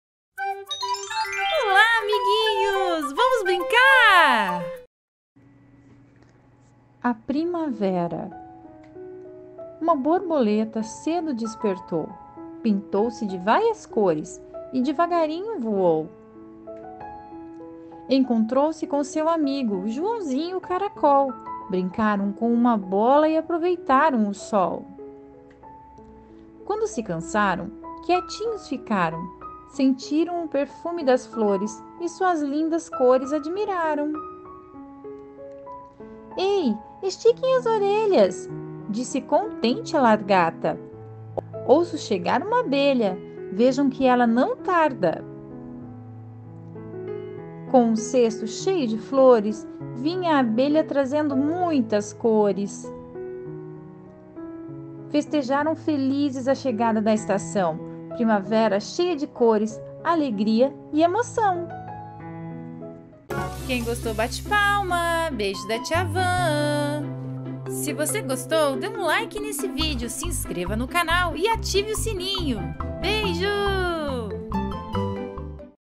A Primavera
A chegada da primavera deixa a todos muito felizes com suas cores e perfumes.